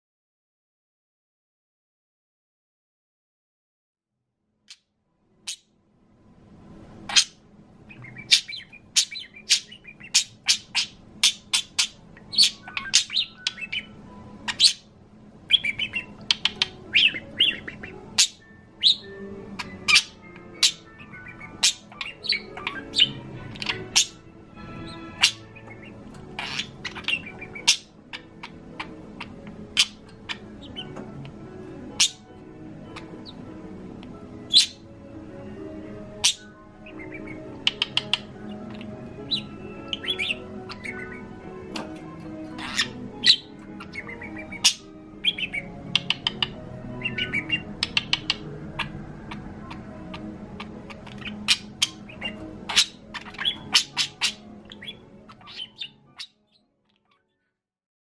Goldie Gets Stroked
Playing with a feisty lady Budgie who ordinarily doesn't like to be touched